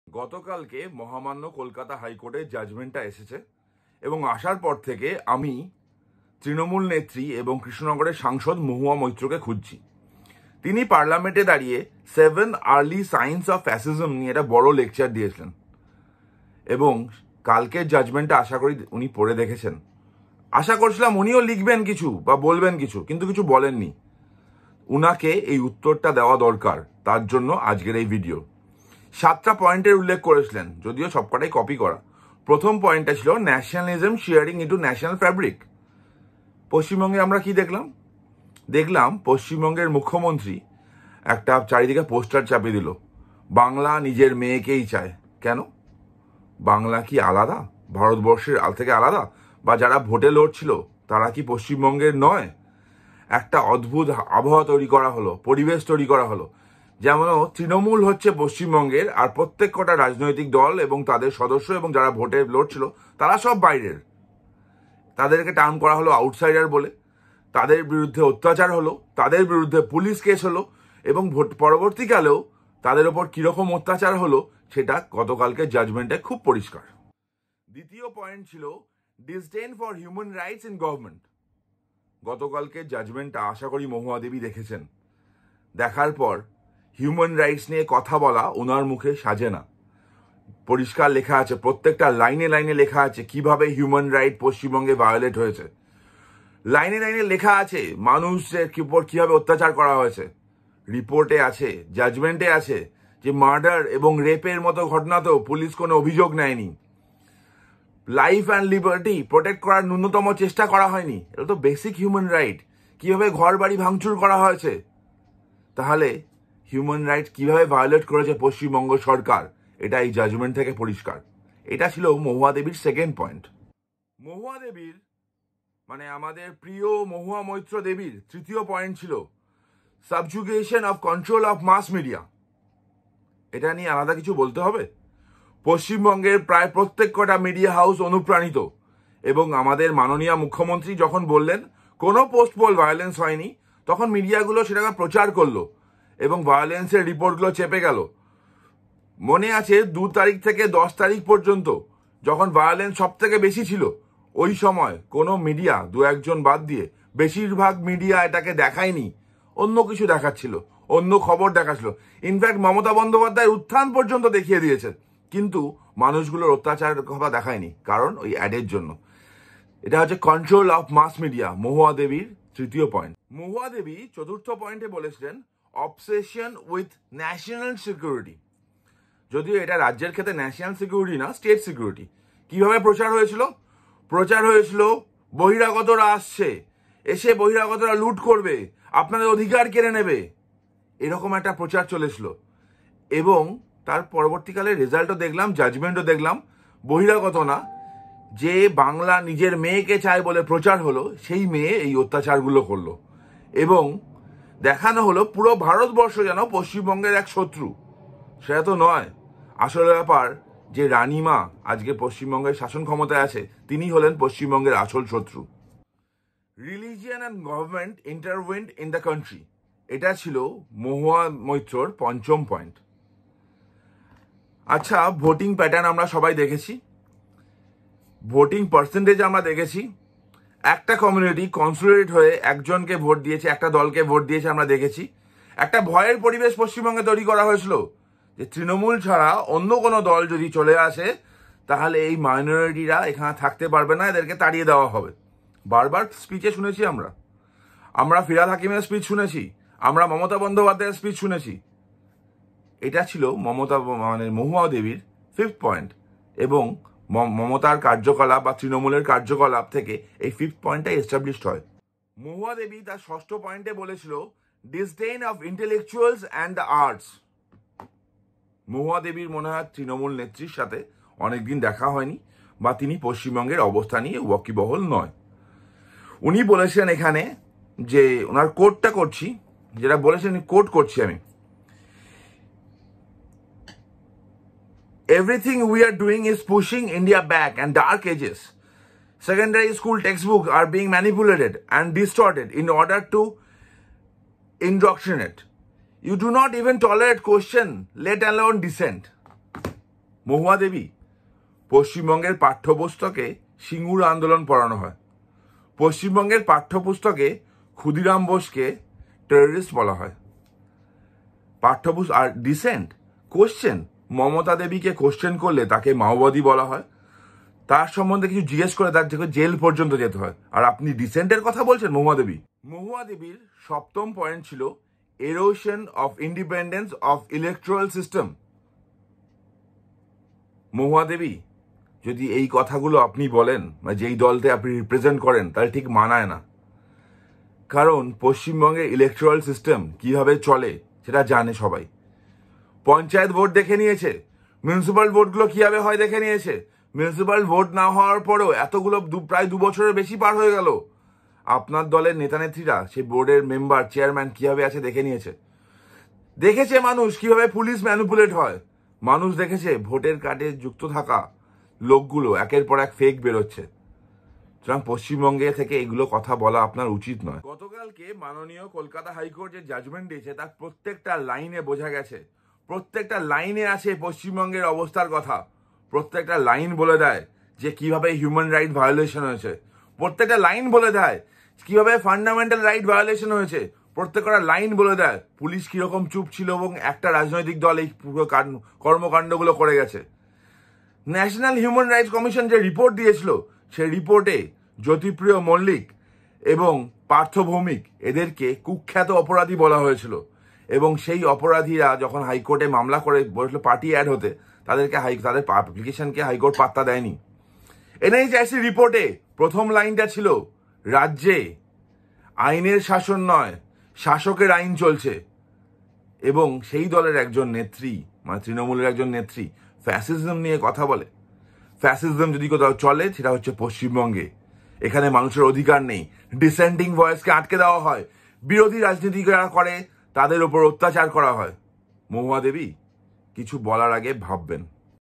7 early Signs of Fascism ? Where are you Mohua Moitra ?
7 early sign of Fascism ?
Mohua Moitra Where are you now ?

1. Powerful and continuing nationalism that is searing into our national fabric:

বাংলা নিজের মেয়েকে চায়, পশ্চিমবঙ্গে অন্য রাজ্যের মানুষ বহিরাগত, অন্য দলের নেতা এবং সদস্যরা পশ্চিমবঙ্গের শত্রু এই বলে বিরোধী দল এদের উপর কিভাবে অত্যাচার হয়েছিল সেটা গতকালকের হাইকোর্টের অর্ডার থেকে পরিষ্কার।

2. There is a resounding disdain for human rights that is permeating in every level of government.

মানবাধিকার নিয়ে তৃণমূলের কোন নেত্রী কথা বলেন? গতকালকের অর্ডারে দেখা গেছে রাজ্য পুলিশ কি করেছে, মানবাধিকার রক্ষার ন্যূনতম চেষ্টা পর্যন্ত করেনি। রাজ্যে আইনের শাসন নয়, শাসকেরা ইন চলছে এটা প্রতিষ্ঠিত।

3. There is an unimaginable subjugation and controlling of mass media today.

ভোট-পরবর্তী হিংসায় যখন পুরো রাজ্য জ্বলছে, মানুষ খুন হচ্ছে এবং তাদের ঘর-বাড়ি জ্বালিয়ে দেওয়া হচ্ছে, এমনকি রেপের ঘটনা পর্যন্ত পশ্চিমবঙ্গের অনুপ্রাণিত মিডিয়া দেখায়নি। শুধুমাত্র বিজ্ঞাপনের লোভে এবং তৃণমূলের আশীর্বাদ পাওয়ার জন্য। পুরো মিডিয়া কন্ট্রোল করে নিয়েছে তৃণমূল।

4. Obsession with national security:

পশ্চিমবঙ্গের যদিও জাতীয় নিরাপত্তার ব্যাপারটা নেই, কিন্তু প্রচার করা হচ্ছে এই বলে যে অন্য রাজ্যের মানুষ পশ্চিমবঙ্গে বহিরাগত এবং এমনভাবে প্রজেক্ট করা হচ্ছে যাতে মনে হয় পশ্চিমবঙ্গ একটা আলাদা দেশ, বিরোধী দল এবং বিরোধী দলের নেতাকর্মীরা পশ্চিমবঙ্গের শত্রু এবং তাদের উপর অত্যাচার করা নরমাল কারণ পুলিশ পদক্ষেপ নেয় না। বাঙালি  অবাঙালি বিভেদ, উচ্চ বর্গ নিম্নবর্গের বিভেদ, বিভিন্ন সময় মমতা বন্দ্যোপাধ্যায় ফিরহাদ হাকিম ইত্যাদি তৃণমূল নেতার বক্তব্য এই পয়েন্টটাকে justify করে।

5. Religion and Government intertwined in the country:

তৃণমূল কংগ্রেস কোন সম্প্রদায় থেকে বেশি ভোট পেয়েছে সেটা দেখলেই এই পয়েন্টটা বোঝা যাবে। একটা গোষ্ঠীকে বোঝানো হলো যে তৃণমূল চলে গেলে তাদের অবস্থা খারাপ হবে। CAA নিয়ে পুরো পশ্চিমবঙ্গে আগুন জ্বালিয়ে দেওয়া হল, সরকারের প্রচ্ছন্ন মদতে। বিশেষ বিশেষ সুবিধা দেওয়া, সংরক্ষণ দেওয়া ইত্যাদির মাধ্যমে প্রতিষ্ঠিত হল যে পশ্চিমবঙ্গ সরকার শুধুমাত্র একটা ধর্মের মানুষের কথা ভাবে।

6. Disdain for intellectuals and the arts:

শিক্ষাব্যবস্থাকে পঙ্গু করে দেওয়া, শিক্ষক নিয়োগ না করা, স্কুল কলেজগুলোর অবস্থা খারাপ করে দেওয়া, স্বঘোষিত বুদ্ধিজীবীদের পয়সা দিয়ে শুধুমাত্র তৃণমূলের সমর্থনে প্রচার করা এবং তৃণমূলের অন্যায়গুলো দেখেও মুখ বন্ধ করে রাখতে বাধ্য করা, এগুলোই চলছে পশ্চিমবঙ্গে।
মমতা বন্দ্যোপাধ্যায় কে প্রশ্ন করা যায় না, বিরোধী রাজনীতির জায়গা পশ্চিমবঙ্গে নেই, বিরোধীরা কোন রাজনৈতিক কার্যক্রম নিলে পুলিশ তাদেরকে আটকে দেয় এবং অ্যারেস্ট করে, স্বৈরাচারী রাজত্ব চলছে পশ্চিমবঙ্গে। রাজনৈতিক আন্দোলন মানে সিঙ্গুর আন্দোলন পড়ানো হয় পাঠ্য পুস্তকে। একটা জাতিকে সম্পূর্ণ অন্ধকারের দিকে নিয়ে যাচ্ছে শুধুমাত্র ভাতা দিয়ে। কর্মসংস্থানের কথা চিন্তা করছে না তৃণমূল সরকার।

7.Erosion of independence in electoral system:

রাজ্য নির্বাচন কমিশনের কথা যত না বলা যায় ততই ভালো, পৌরসভা ভোট, পঞ্চায়েত ভোট আমরা দেখেছি।
শুধুমাত্র নির্বাচন কমিশন কেন? পশ্চিমবঙ্গের কোন কমিশন অনুপ্রাণিত নয়? প্রত্যেকটা কমিশন কাজ করে মমতা বন্দ্যোপাধ্যায়ের কথামতো।

গতকালকে মহামান্য কলকাতা হাইকোর্ট order দিয়েছেন। প্রত্যেকটা পাতা বর্ণনা করে পশ্চিমবঙ্গের অবস্থা। প্রত্যেকটা পাতা প্রমাণ করে যে পশ্চিমবঙ্গে গণতান্ত্রিক অধিকার এবং মৌলিক অধিকার বলে কিছু নেই।